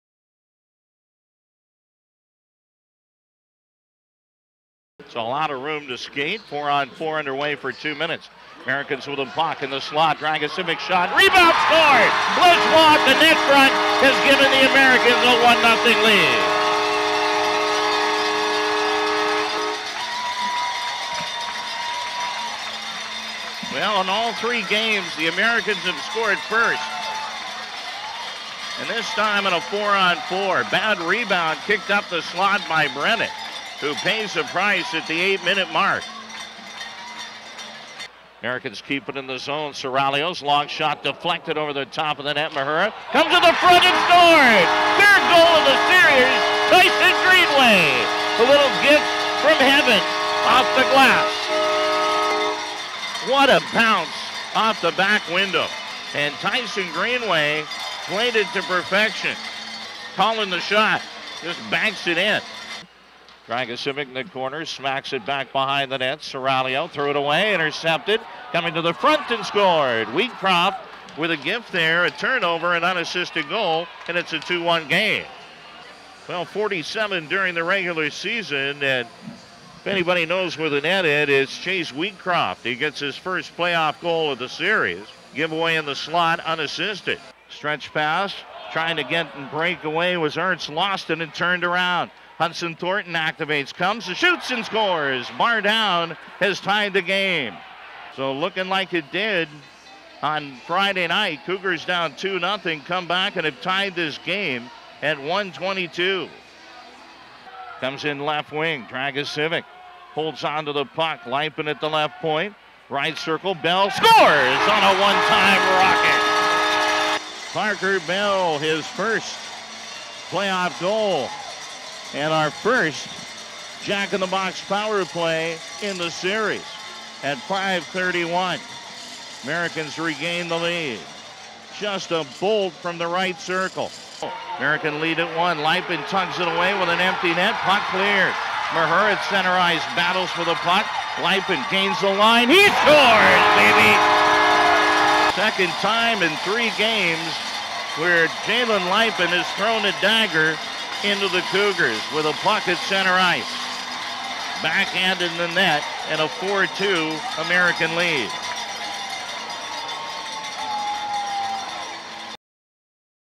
WHL Playoffs Highlights: Cougars (2) at Americans (4) - April 4, 2023
Highlights from Game 3 of the best-of-seven First Round series between the Prince George Cougars and Tri-City Americans.

Feeling social?